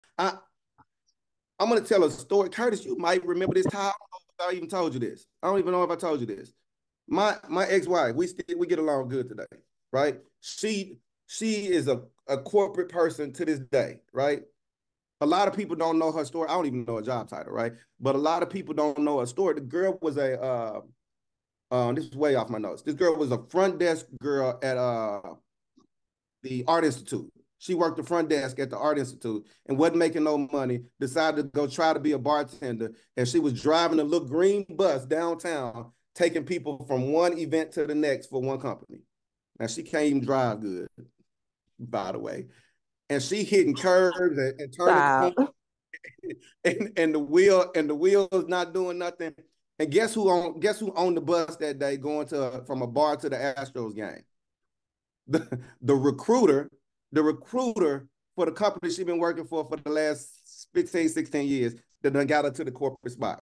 From Art Institute Front Desk Girl to Corporate Success Story: A Journey of Determination
Join me as I share the inspiring story of my ex-wife's transformation from a struggling front desk girl to a successful corporate professional. Discover how her decision to become a bartender and a hilarious bus ride turned her life around. Get ready to be motivated and inspired!